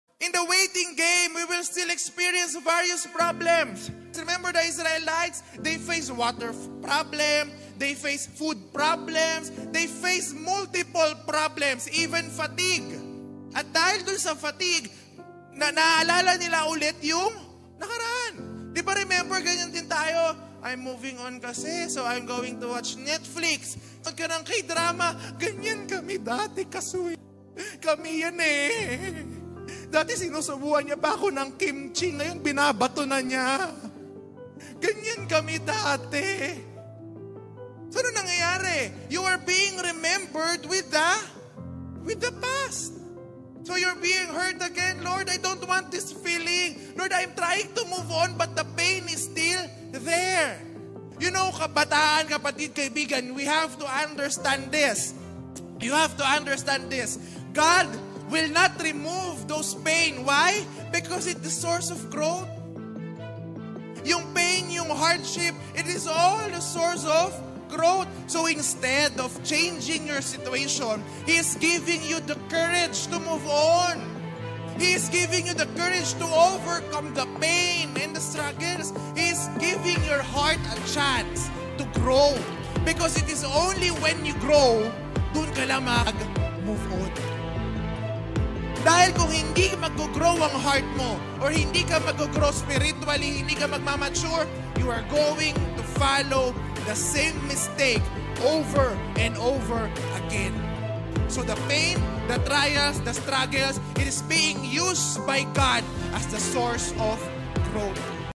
If we will not grow and move on, we will just follow the same mistake over and over again. 🔥
Mahirap bang mag-Move on?

Ano mang klase ng pag-move on yan, mapa wrong realtionship, past mistakes, hurtful events, and even sa kasalanan at maling sistema na nakasanayan mo na. MASAKIT AT MAHIRAP MAG MOVE-ON!

But we cannot enter our promised land if we will not move on sa mga bagay na pinalaya na tayo ng Panginoon!

"God is not removing the pain because it is the source of growth that will make our hearts move on from the wrong desire and perspective."

If we will not grow and mature, we will just follow the same mistake over and over again.

📷 JLH Youth Fellowship